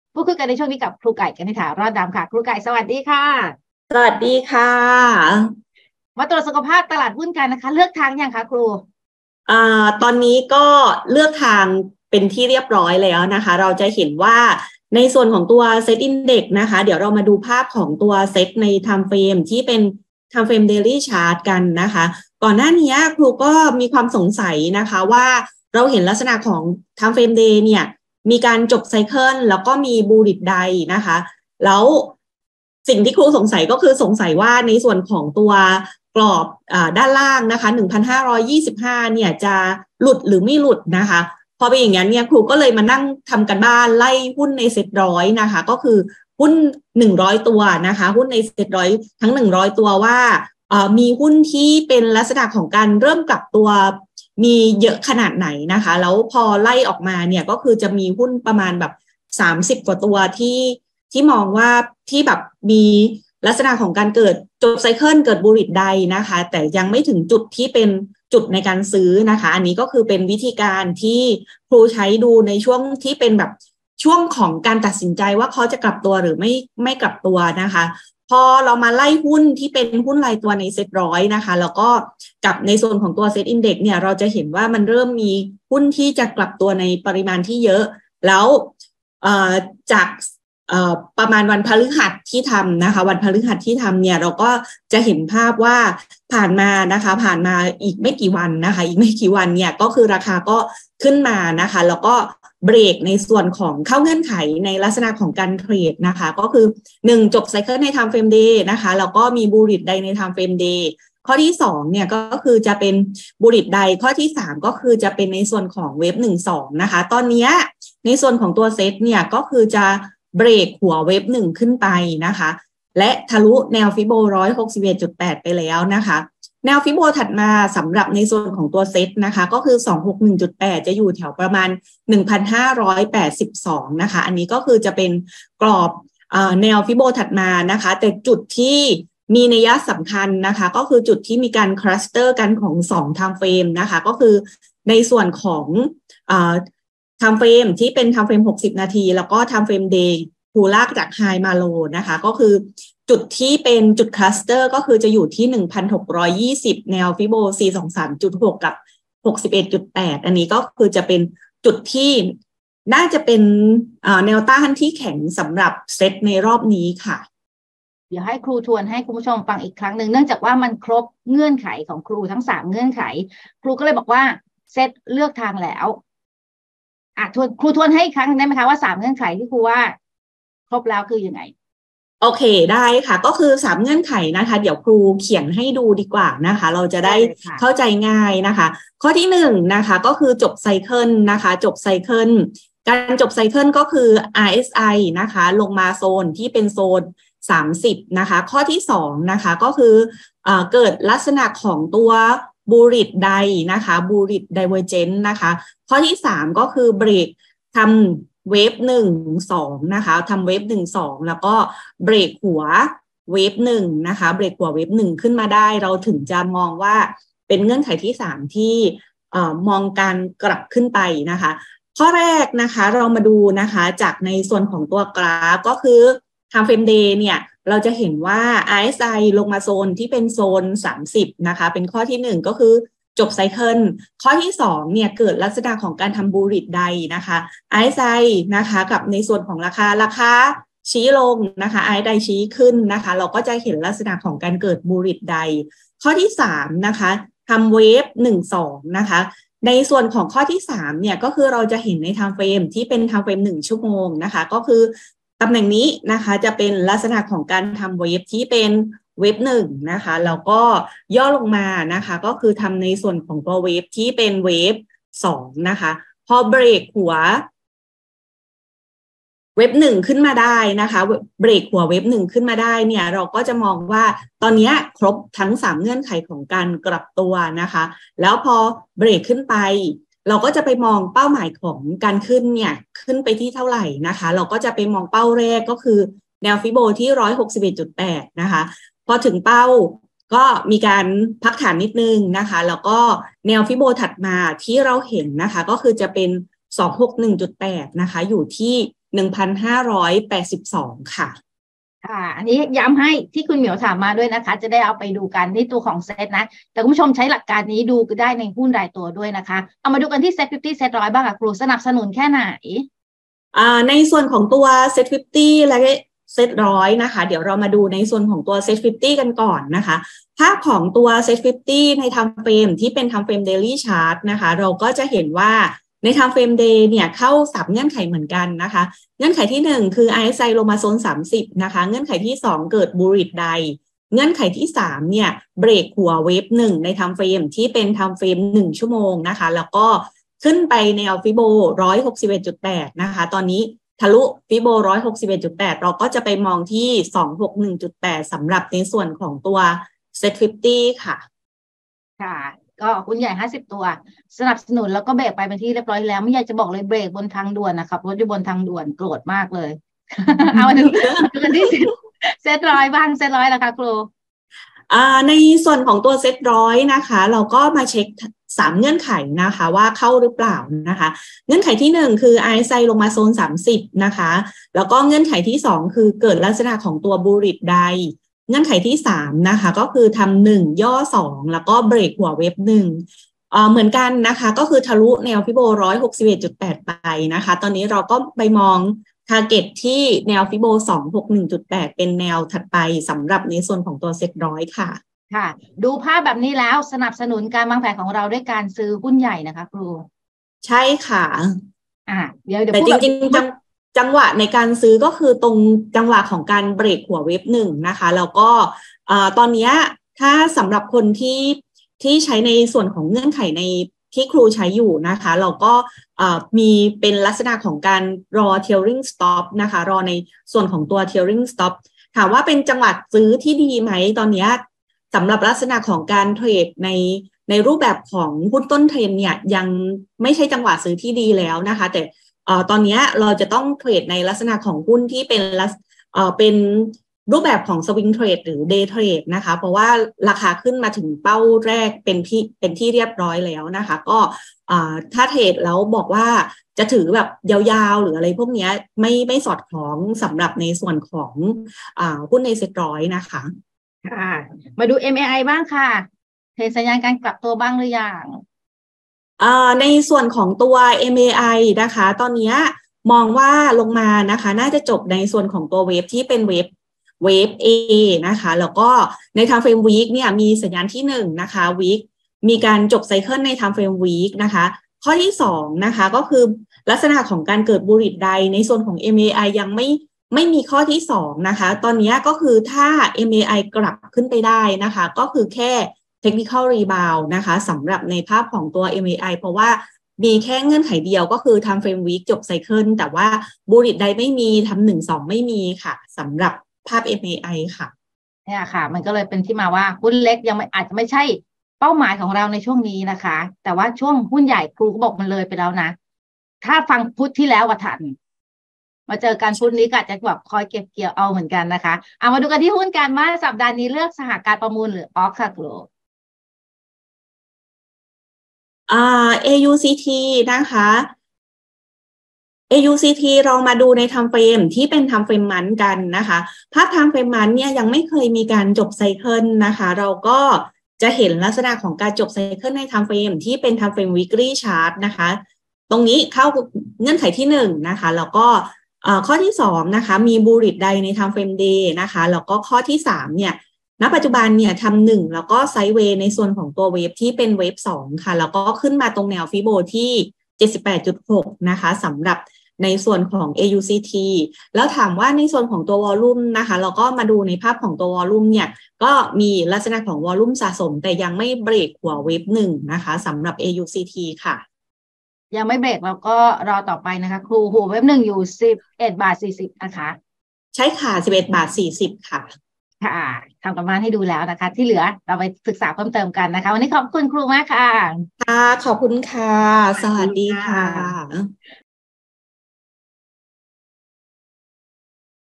สำรวจตลาดทำการบ้านหุ้น AUCTกับครูไก่ กนิษฐา รอดดำ I TNN WEALTH 14 มิ.ย. 66
- "สำรวจตลาดทำการบ้านหุ้น AUCT"
กับครูไก่ กนิษฐา รอดดำ Fulltime Trader

ติดตามข่าวหุ้นและการลงทุนทางไลน์
ติดตาม TNN Wealth ผ่านช่องทางต่าง ๆ ได้ที่